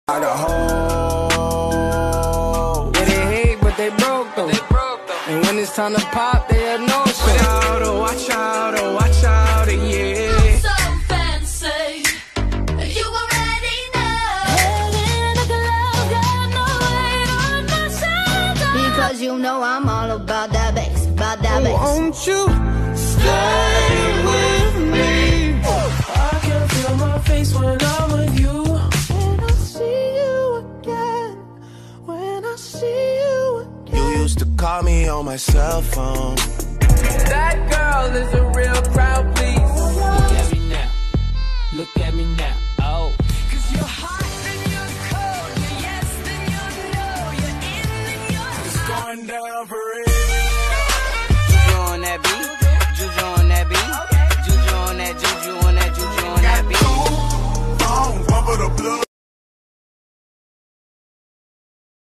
Trash vs Fire Tik Tok Sounds (Jersey boys edition) REPOST FROM OLD ACCOUNT
So 'Harjyot Kaur' is my old account and today the video hit 1 million+ and like I'm screaming atm. This video was in 2020 like right in the beginning and it brings back so many memories. I love you guys for supporting my new account [this one] and thanks for making my day with sweet comments.